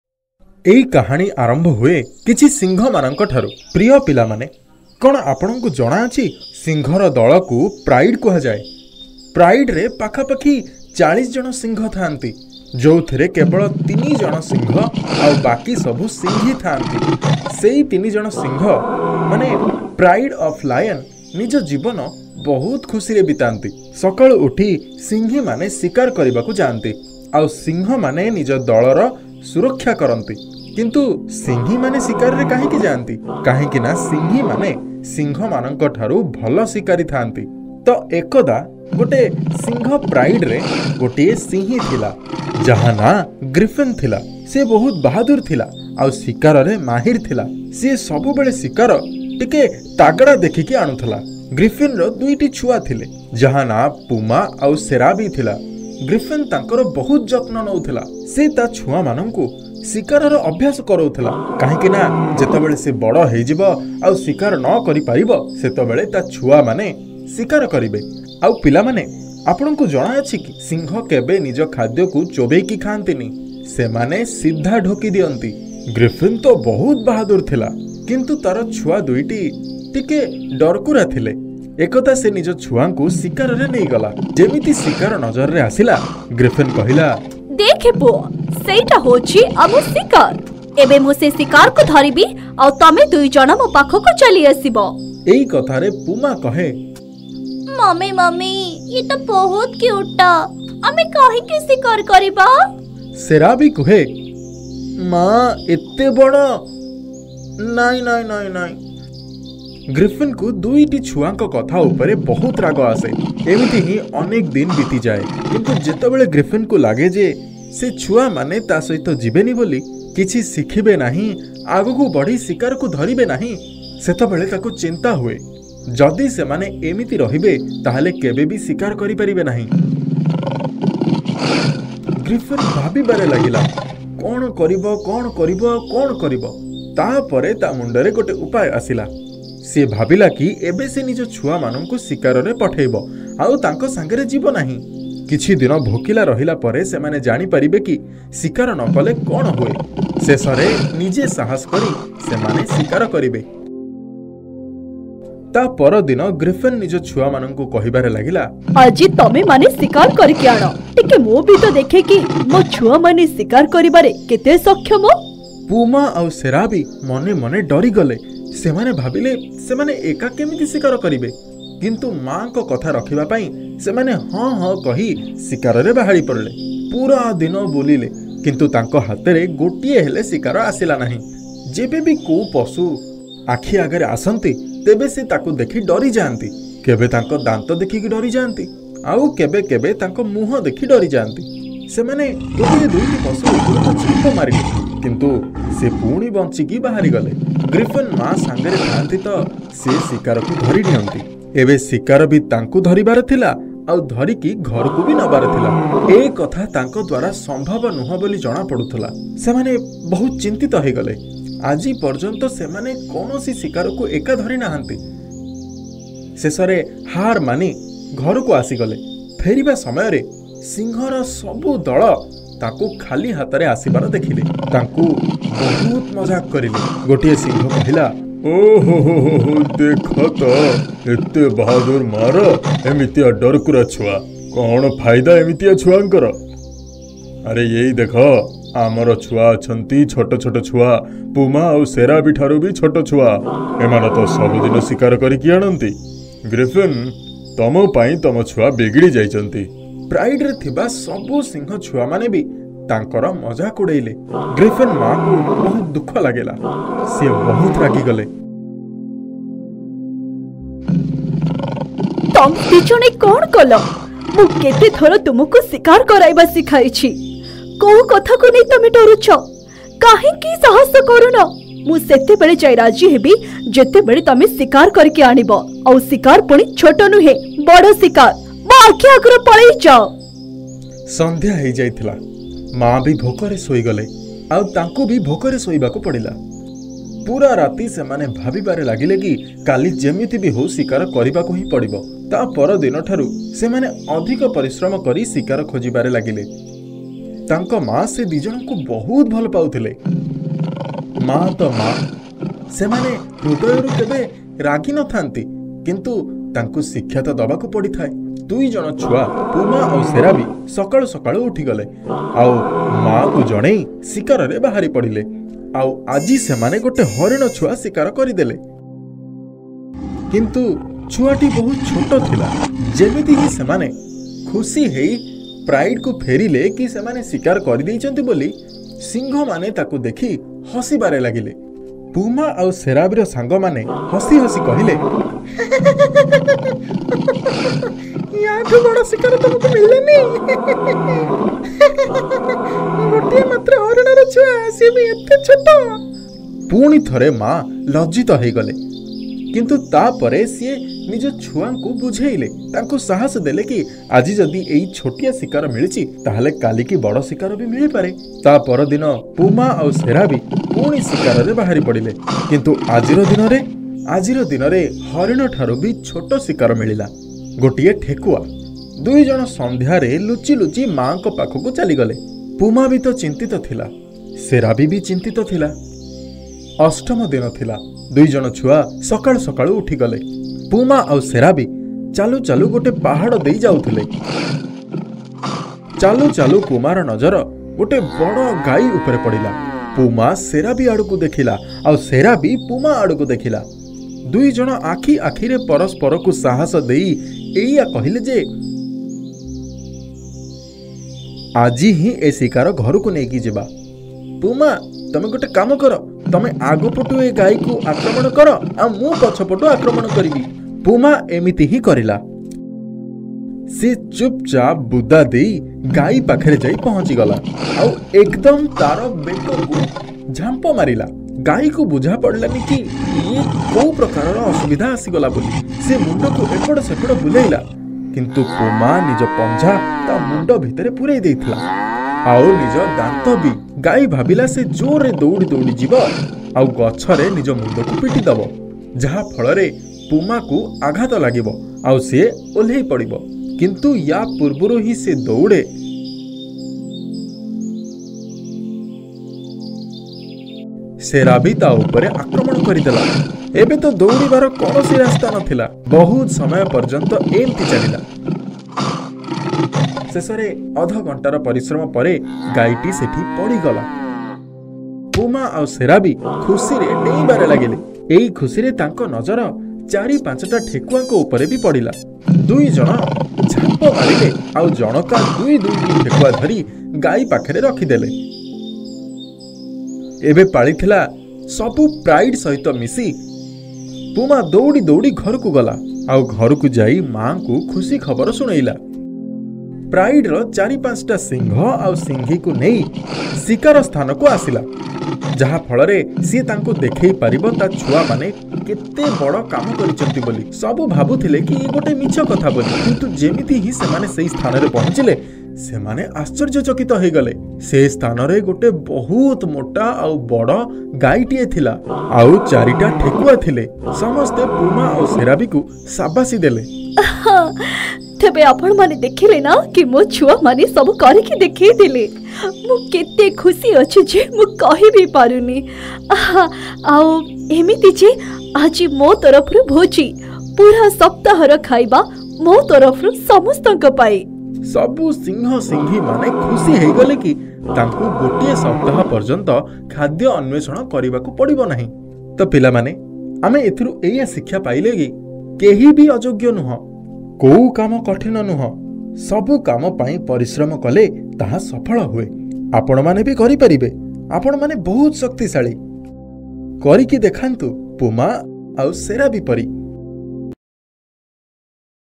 କଠିନ ପରିଶ୍ରମର ପୁରସ୍କାର The Reward of Hard Work - ଆମେ ଯେତେ କଠିନ ପରିଶ୍ରମ କରିବୁ, ଫଳାଫଳ ସେତେ ଭଲ
କାହାଣୀ କିଛି ସିଂହ ସହିତ ଆରମ୍ଭ | ପ୍ରିୟ ପିଲାମାନେ ଜାଣନ୍ତି କି, ସିଂହମାନଙ୍କର ଏକ ଦଳ 'ଗର୍ବ' ଭାବରେ ଜଣାଶୁଣା | ଅହଂକାରରେ ଚାଳିଶ ସିଂହ ଅଛି, ଯେଉଁଥିରେ ତିନୋଟି ସିଂହ ଏବଂ ଅବଶିଷ୍ଟ ସମସ୍ତ ସିଂହୀ ଅଟନ୍ତି | ସିଂହମାନଙ୍କର ଗର୍ବ ଶାନ୍ତିପୂର୍ଣ୍ଣ ଜୀବନଯାପନ କଲା।